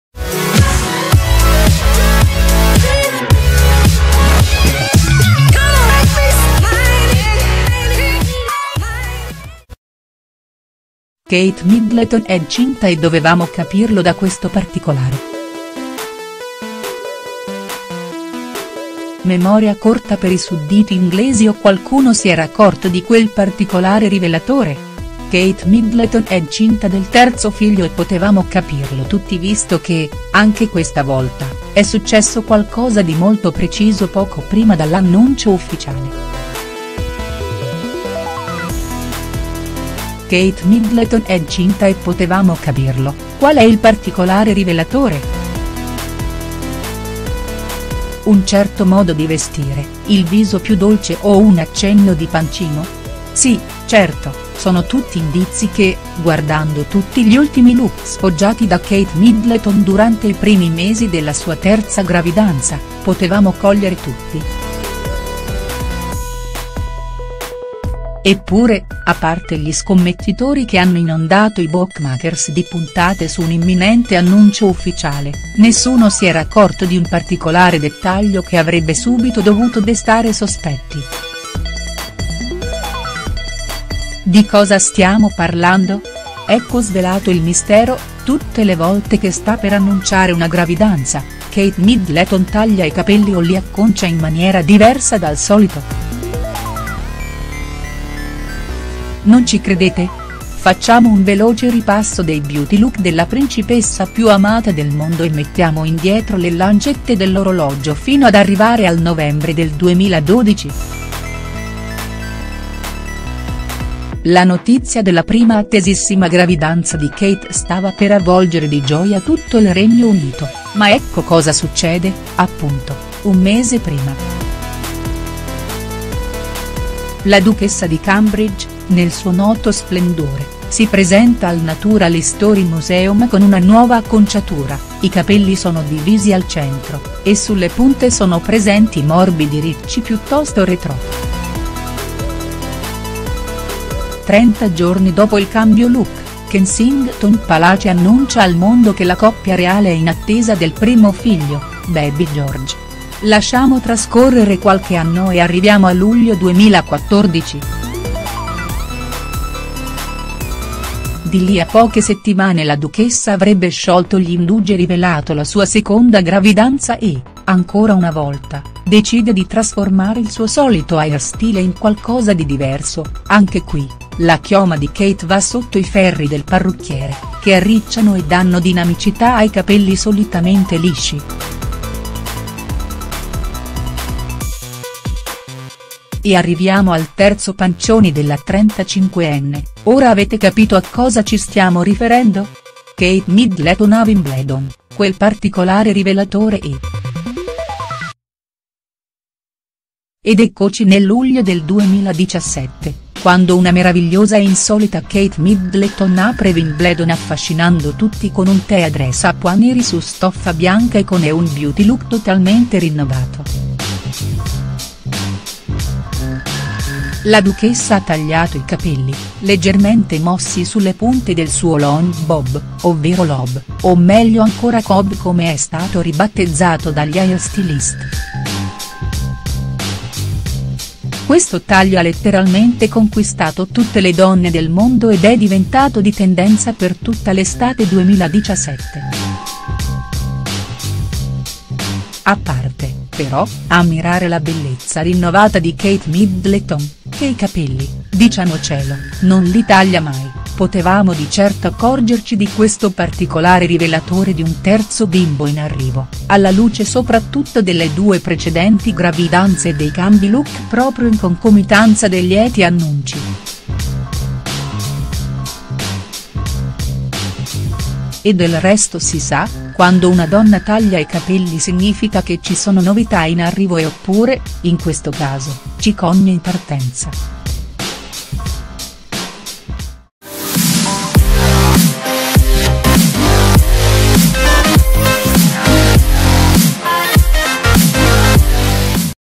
Kate Middleton è incinta e dovevamo capirlo da questo particolare.HD
C’è un particolare preciso da cui potevamo capire che Kate Middleton era incinta del terzo figlio: ogni volta che è in dolce attesa, puntualmente si verifica questa circostanza piuttosto singolare. Dovevamo notarlo subito ma, forse, non tutti si ricordavano delle due gravidanze precedenti.